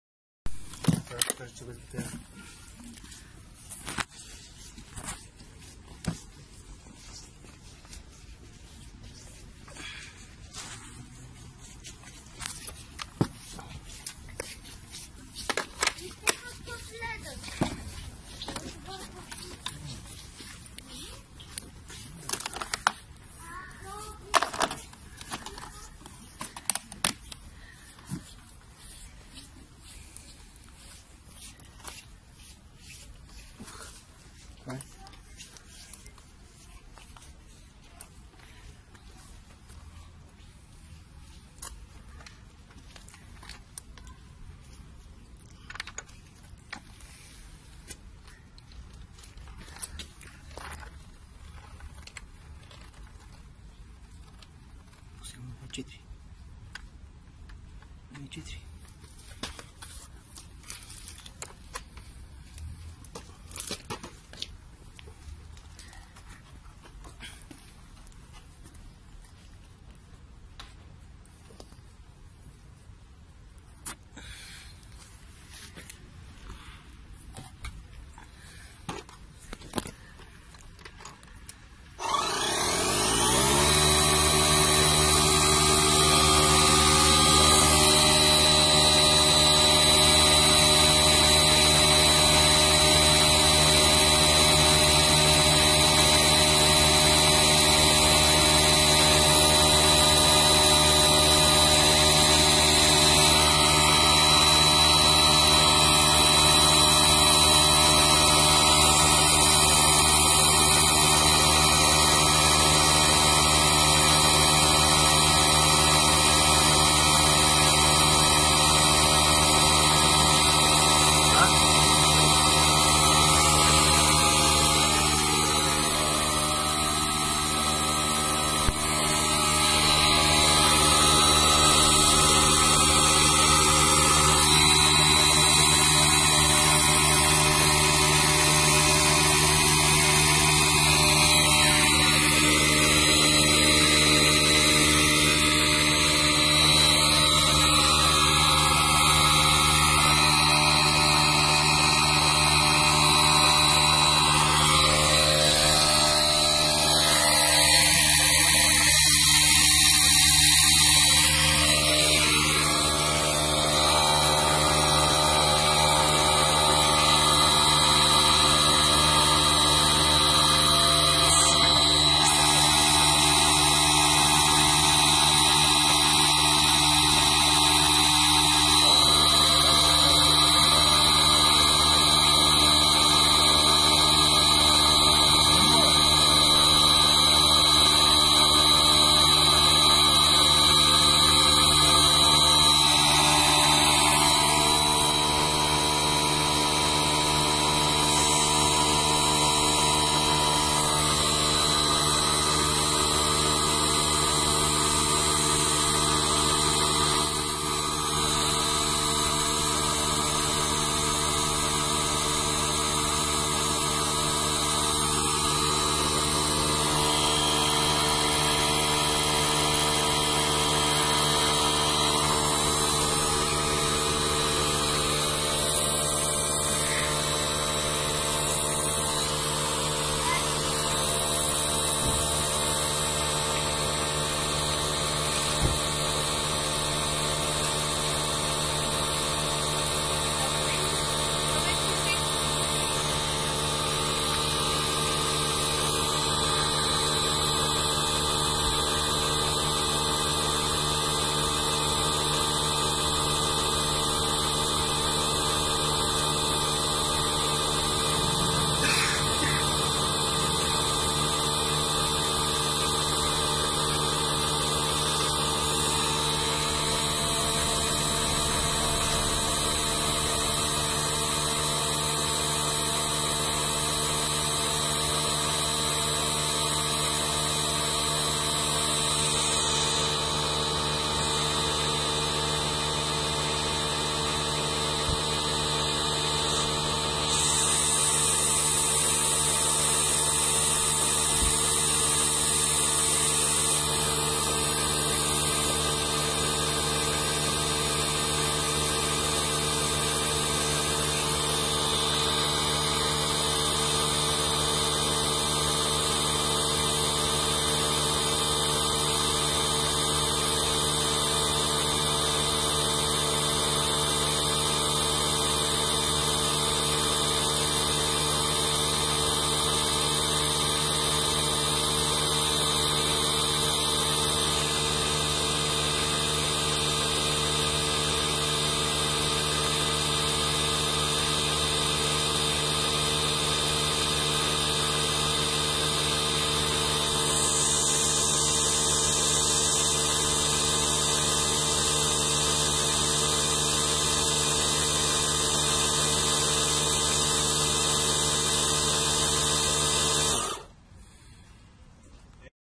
Акумулаторен компресор DeWalt DCC018N-XJ - 18V/11бара
- Лек и компактен безжичен компресор за безжично използване на инструменти за сгъстен въздух
- Компресорът се захранва с литиево-йонна батерия с изходящо напрежение 18 V
- LED работно осветление за работа на неосветени пространства
- Идеален за справяне с големи задачи благодарение на изхода на високо / ниско налягане
- С 3 източника на енергия може да се захранва от електрическата мрежа у дома или чрез 12-волтова връзка (автомобил) и дори безжично използване с 18-волтова литиево-йонна батерия
- Снабден с дигитален дисплей и автоматично изключване при достигане на предварително зададената цел за налягане
- Функция за автоматично изключване предпазва напомпваните детайли от повреда
- Доставя се без батерия и зарядно устройство